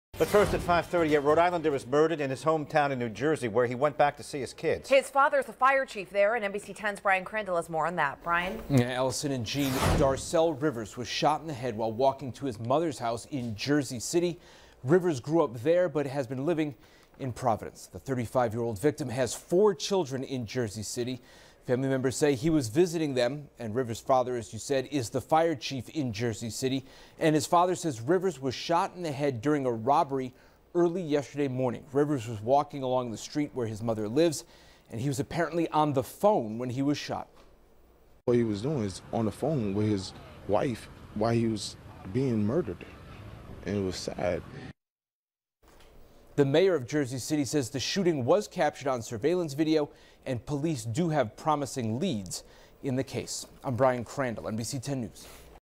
RI man visiting kids in NJ shot dead
A Providence man who is the son of Jersey City's fire chief is fatally shot during a robbery.